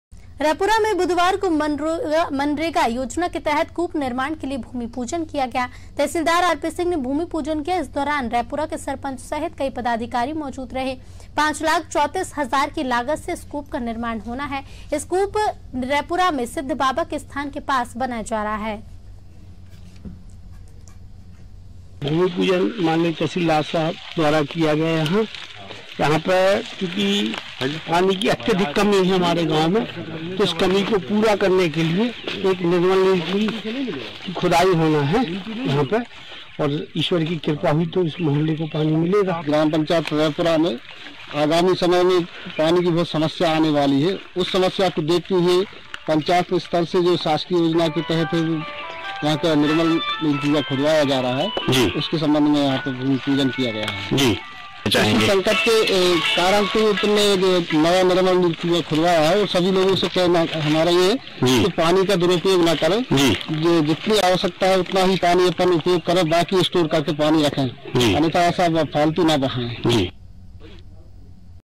रैपुरा : मनरेगा के तहत विकास की सौगात |Top News | Headlines | Breaking News
रैपुरा में बुधवार को मनरेगा योजना के तहत कूप निर्माण के लिए भूमिपूजन किया गया. तहसीलदार आरपी सिंह ने भूमिपूजन किया. इस दौरान रैपुरा के सरपंच सहित कई पदाधिकारी मौजूद रहे 5 लाख 34 हज़ार की लागत से इस कूप का निर्माण होना है. यह कूप रैपुरा में सिद्ध बाबा के स्थान के पास बनाया जा रहा है. ।